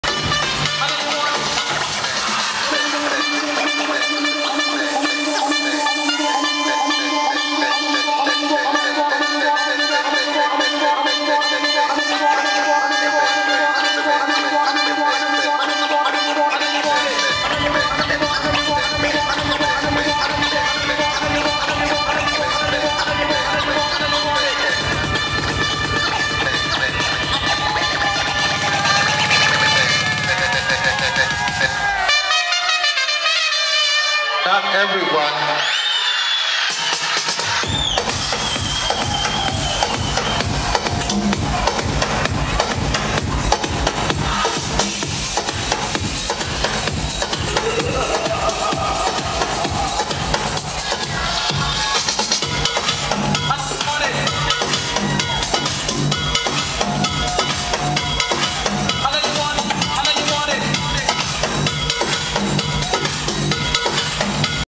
Club Fratelli presents Greg Stainer
Greg Stainer rocking Club Fratelli Dubai (warm up by DJ Rubber)